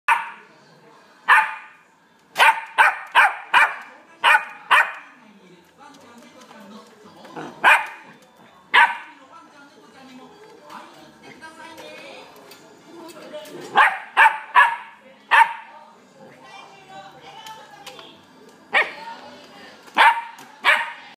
ペットショップCoo&RIKU No291495 Wコーギー
生年月日：2016年3月27日
性別：オス
カラー：セーブル＆ホワイト